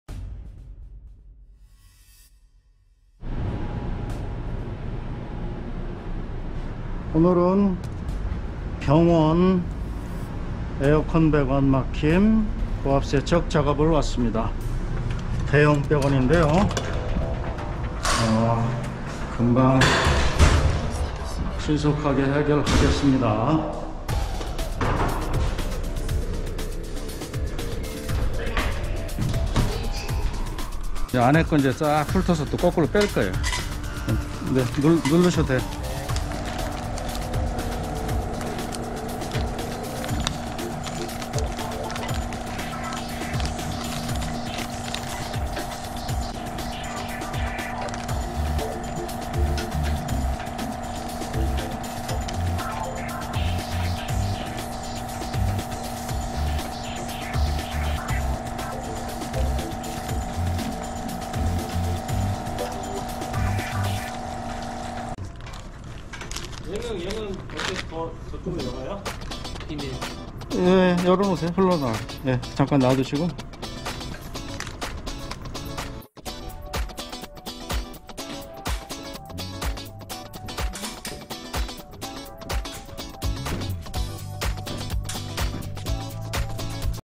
에어컨 드레인 배수관막힘 고압세척으로 시원하게 통수완료 했어요 923화
안녕하세요 배관막힘 최종해결사 세계최고 수준의 장비와 기술을 보유한 고압세척 파워클린 입니다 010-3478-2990